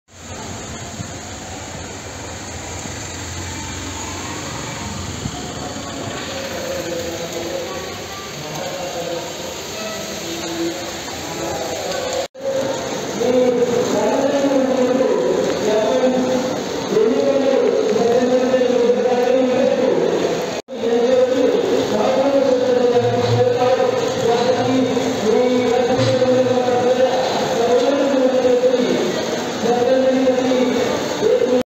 BARAMATI | अनेक दिवसांच्या प्रतिकशे नंतर बारामती परिसरात पाऊस
-अनेक दिवसांच्या विश्रांतीनंतर बारामती शहर आणि परिसरात संतत धार पावसाला सुरुवात झाली. पावसाने अचानक हजेरी लावली. अचानक आलेल्या पावसामुळे नागरिकांची तारांबळ उडाली गेल्या अनेक दिवसांपासून बारामती शहर आणि परिसरात  पावसाने ओढ दिली होती. मात्र कालपासून वातावरणात बदल झाला,असून सकाळपासून ढगाळ वातावरण होते त्यातच संतत धार पावसाने हजेरी लावली. या संतदार पावसा मुळे  शेतातील उभ्या पिकांना मोठा दिलासा मिळणार आहे.